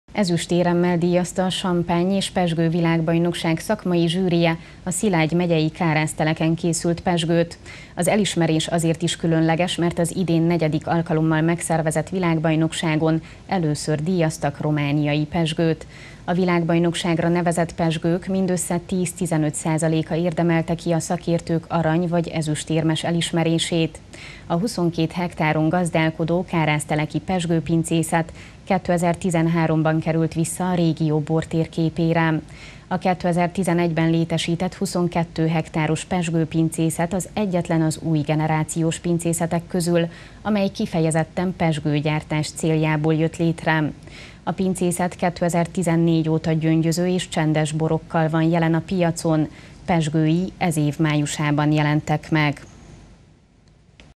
Szilágysági pezsgőt díjaztak – Erdélyi Magyar Televízió
Ezüstéremmel díjazta a Champagne és Pezsgő Világbajnokság szakmai zsűrije a Szilágy megyei Kárászteleken készült pezsgőt. Az elismerés azért is különleges, mert az idén negyedik alkalommal megszervezett Világbajnokságon először díjaztak romániai pezsgőt.